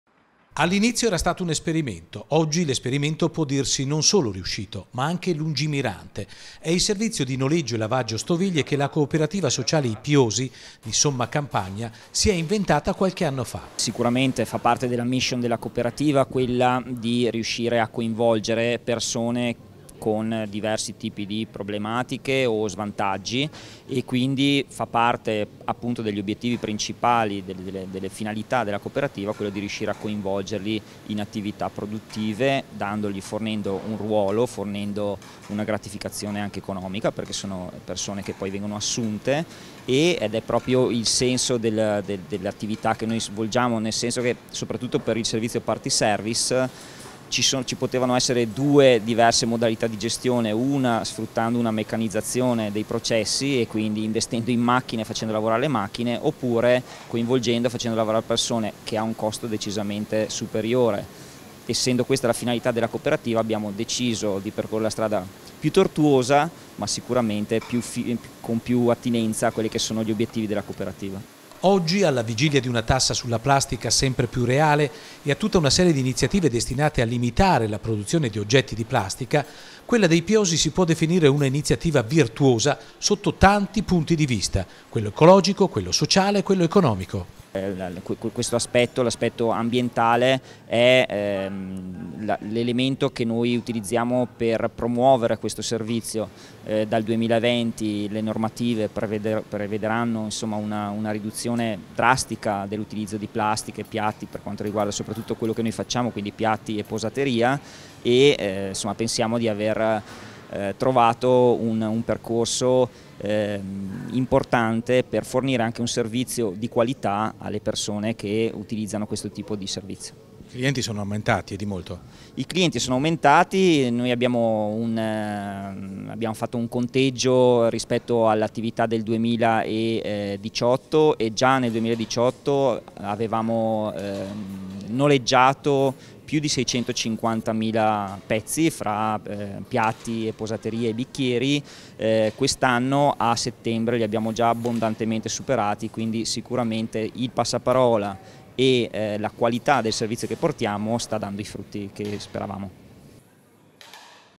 Alternativa alla Plastica
All'inizio era stato un esperimento oggi l'esperimento può dirsi non solo riuscito ma anche lungimirante. E' il servizio di noleggio e lavaggio stoviglie che la Cooperativa Sociale “I Piosi” di Sommacampagna si è inventata qualche anno fa.

LUIGI MARTARI
Dir. Coop. Sociale “I Piosi” Sommacampagna VR